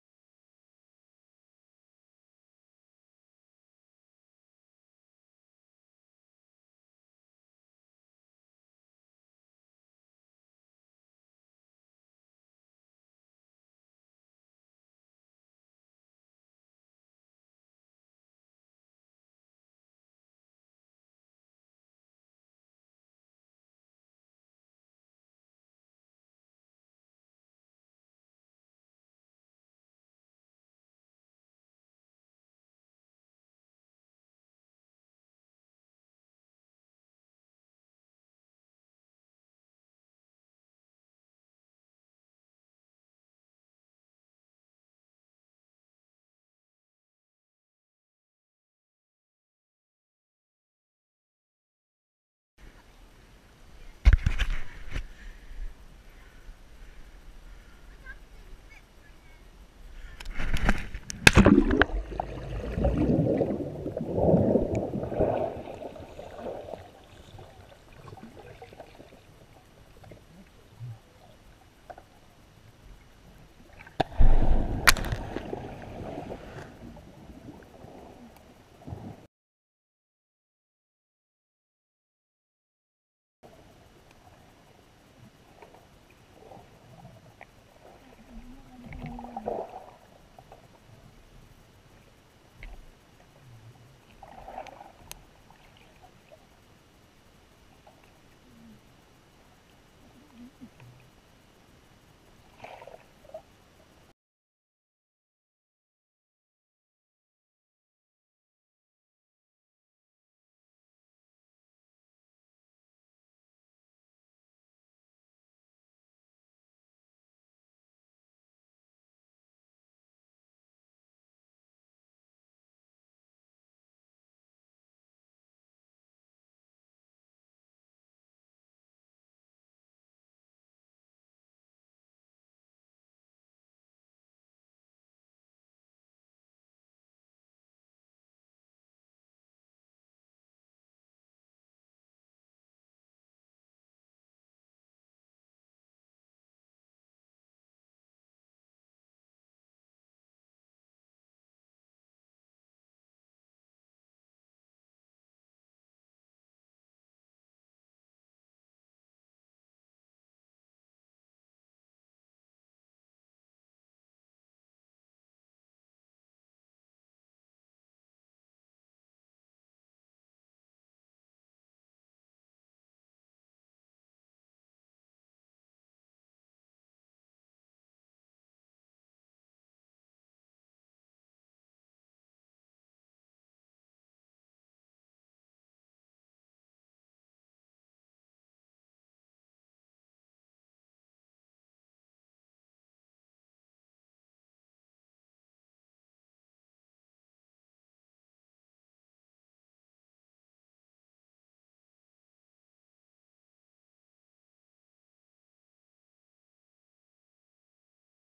The day we caught a GoPro fishing
So this happened.. We decided to go fishing and with the first cast of the night my sisters (Madeline Rose) fishing rod pulls up a GoPro camera! The date of the original footage was 13/08/15 and it was found on 05/07/16.